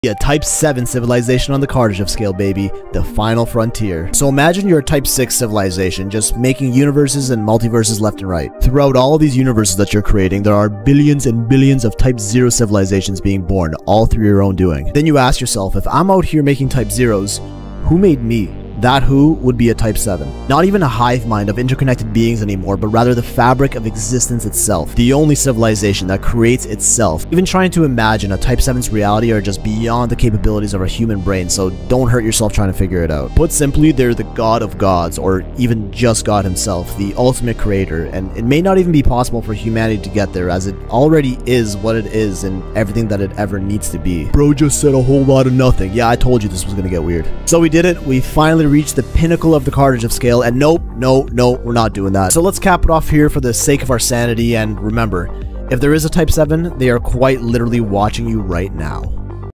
What is a Type 7 civilization on the Kardashev scale? They ARE everything
The absolute (last?) stop on the extended Kardashev Scale - a Type 7 civilization is where it starts getting weird... like really, hypothetical, philosophical, mind-bending weird.

It probably won't make sense, and that's just fine.